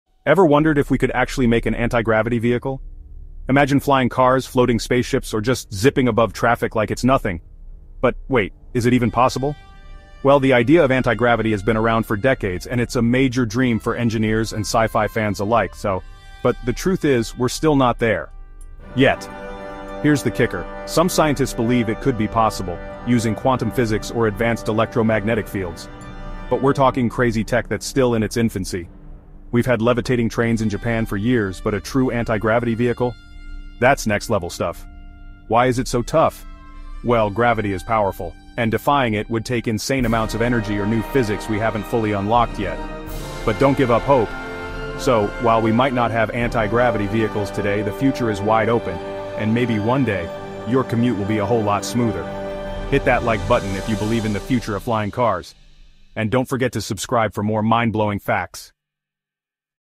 Anti-Gravity Vehicles: Sci-Fi or Reality? 🚀
What if your daily commute could defy gravity? 🚀 In this YouTube Short, we dive into the thrilling world of anti-gravity vehicles! From flying cars to floating spaceships, the dream of flying above traffic is closer than you think—at least in theory! 🤯

Join us as we explore the scientific concepts behind anti-gravity, including the cutting-edge technologies like quantum physics and electromagnetic fields that could one day revolutionize transport. 🌍💡 While we’re not there yet, the future holds endless possibilities!

Do you believe in the future of flying cars? Let's keep the conversation going!